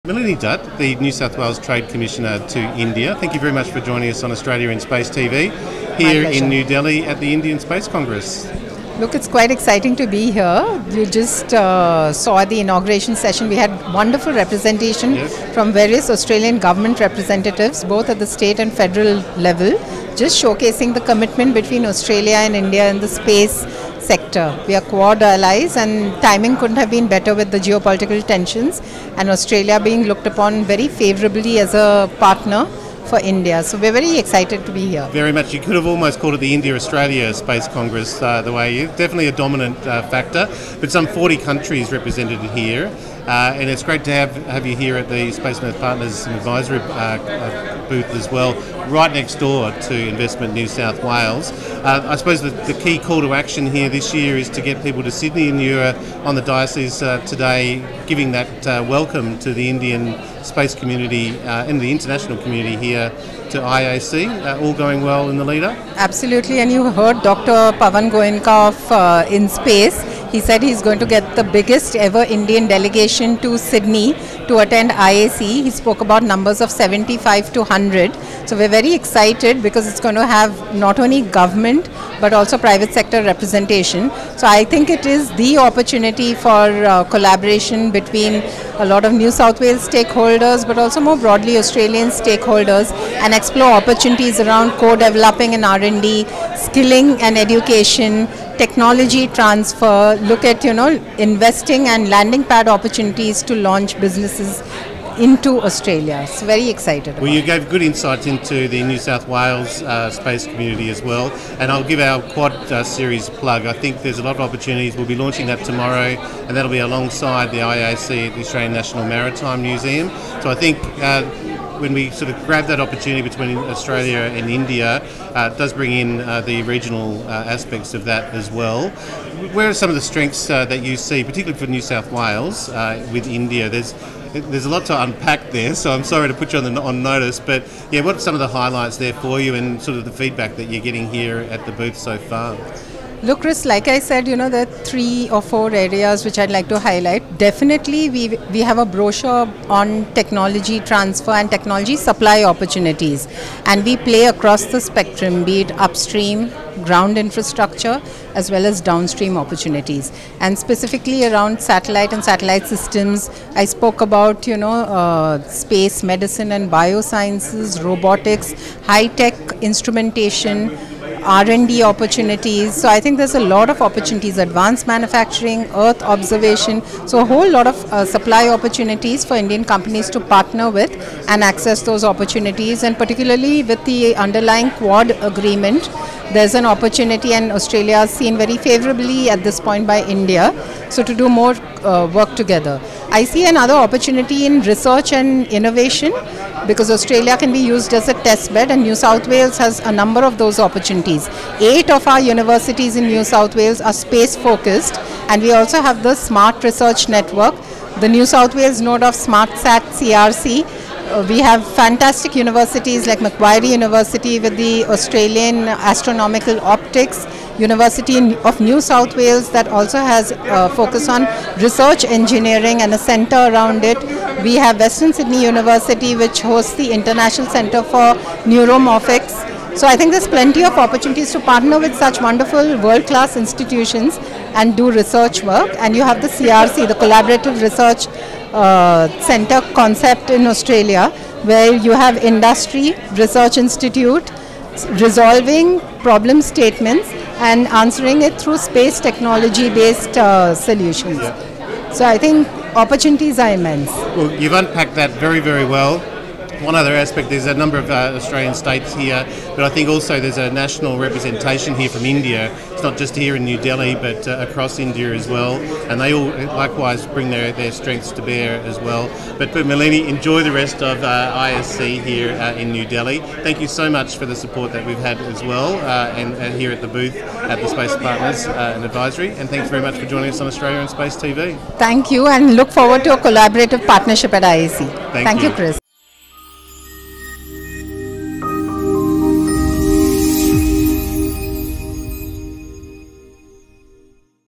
Investment NSW issues invitation to IAC, 2025 at the India Space Congress
We speak with Malini Dutt, Investment and Trade Commissioner - India, Investment NSW at the India Space Congress, 2025, New Delhi.

Investment NSW were Country Sponsor at the ISC2025, to demonstrate intent and commitment that Australia and India are trusted partners and true QUAD allies.

Investment NSW encouraged the Indian space industry to attend the International Astronomical Congress 2025, IAC 2025 Sydney, co-hosted by NSW government and Space Industry Association of Australia.